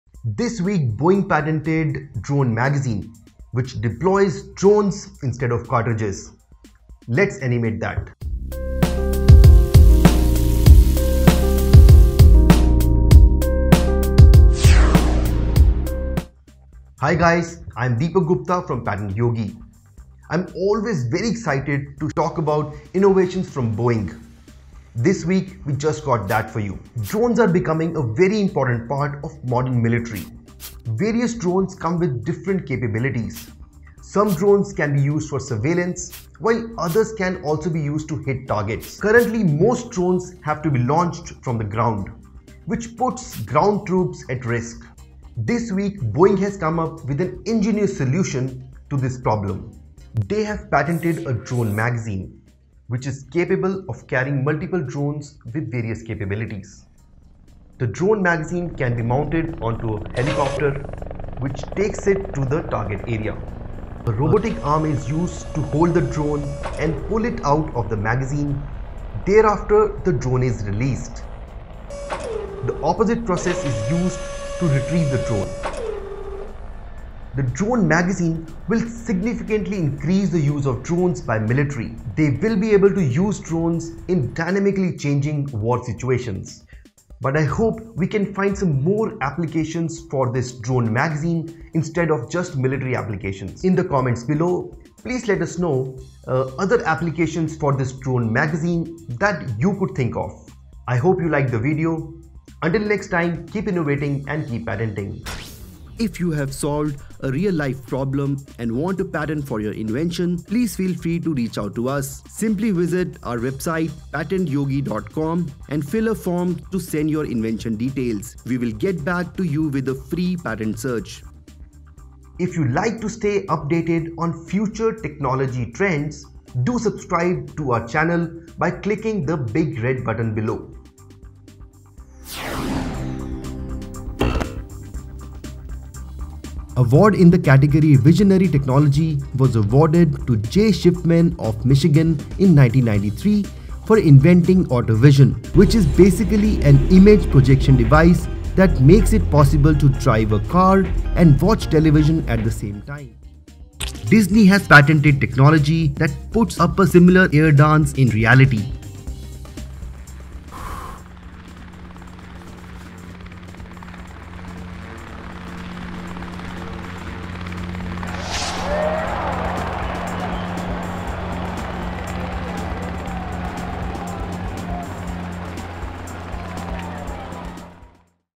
Boeing patented Mothership for drones
This week Boeing patented a drone magazine, which deploys drones instead of cartridges.

I am always very excited to show innovations from Boeing. Today we have just that.

Drones are becoming an important part of modern military.

Various drones have different capabilities, some drones can be used for surveillance, while some other drones can be used to even hit targets.

Currently, these drones are launched from ground, which puts the ground forces at risk.

Boeing has come up with an Ingenious solution. They have patented a drone magazine, which stores multiple drones with different capabilities. The drone magazine can be mounted on a helicopter, which carries the drones near the target area.

A robotic arm is used to hold the drone and pull it out of the magazine.  Thereafter, the drone is released. The opposite process is used to retrieve the drone.

The drone magazine will significantly increase the use of drones by the military. They will be able to use drones with dynamically changing mission objectives.

I hope we can find some other uses of the drone magazine apart from the military applications.

Patent Information
Patent Number: 9,132,916
Patent Title: Aircraft deployment and retrieval of unmanned aerial vehicles
Inventors: Hanna; Mark (Medford, NJ), Poling; David R. (West Chester, PA)
Assignee: The Boeing Company (Chicago, IL)
Family ID: 49485526
Appl. No.: 13/664,323
Filed: October 30, 2012